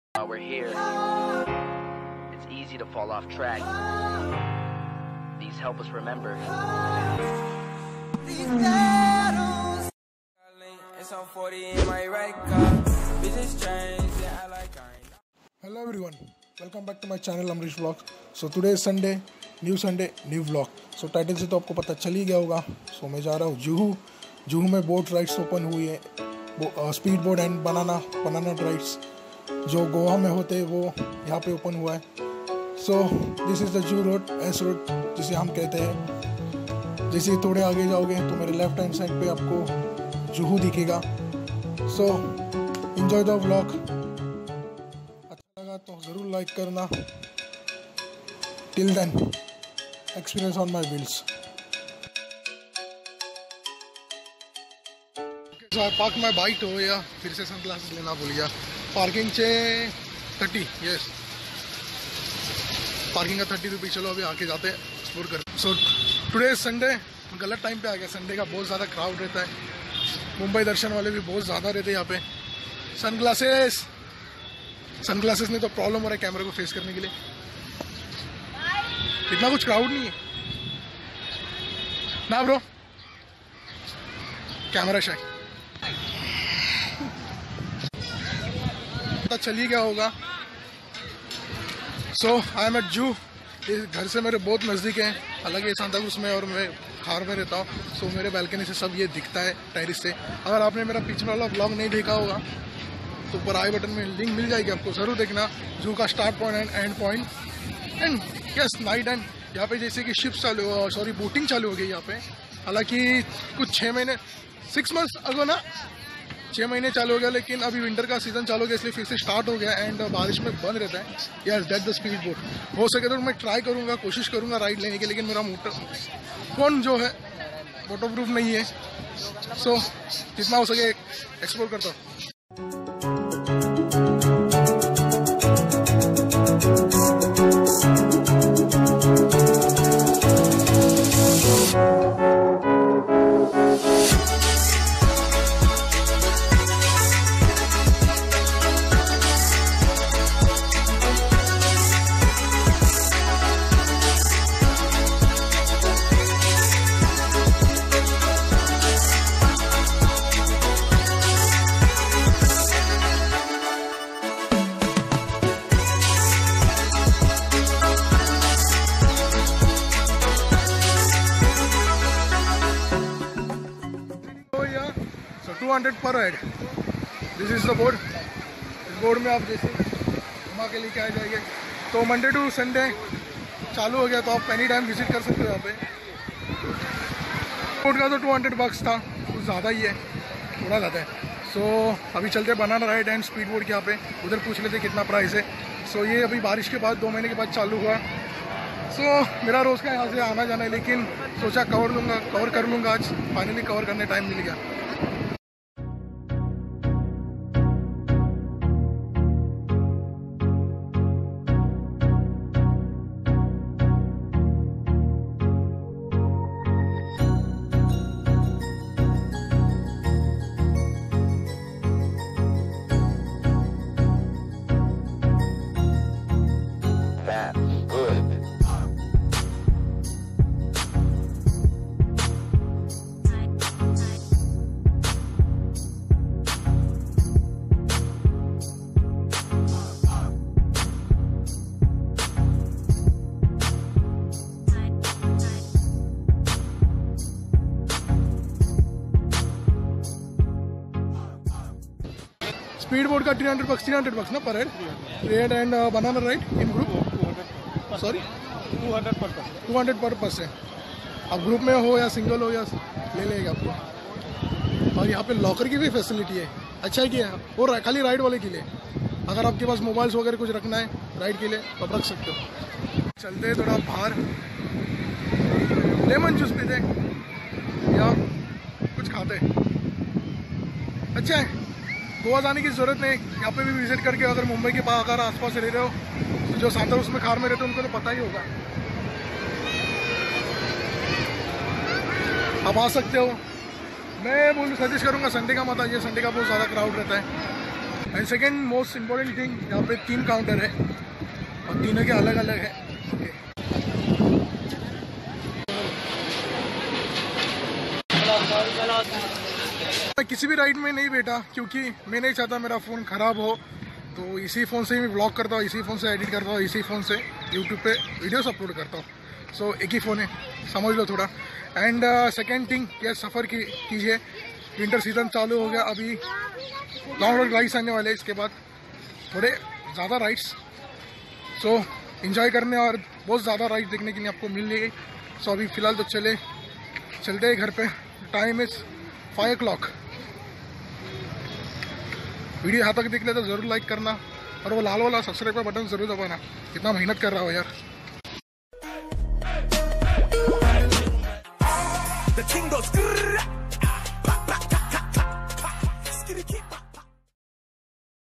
Juhu Beach New Boat RIDES explore
In this you will come to see Boat RIDE which are STARTED at Juhu

Do enjoy like COMMENT SHARE

INSTAGRAM AMRISH VLOG